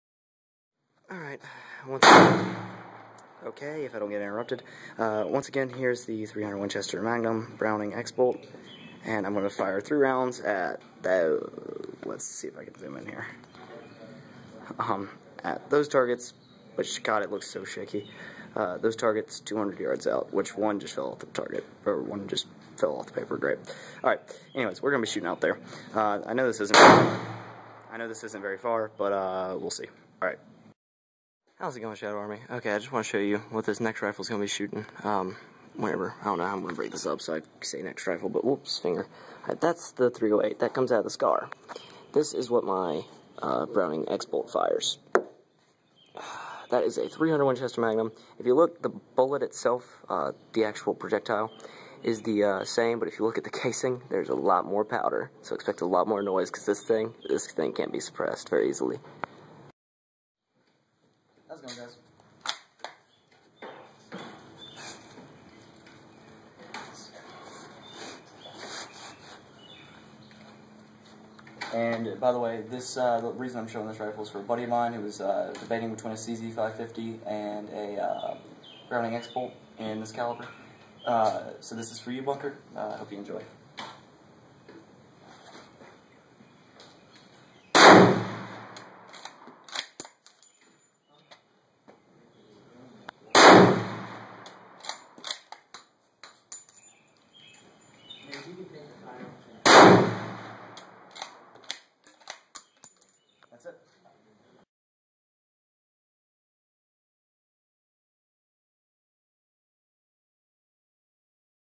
Browning A-Bolt .300 Winchester Magnum
Took this rifle for myself, but recorded it for you guys and my friend. Sorry I've been away for a week. Had friends over and needed a bit of a break. c: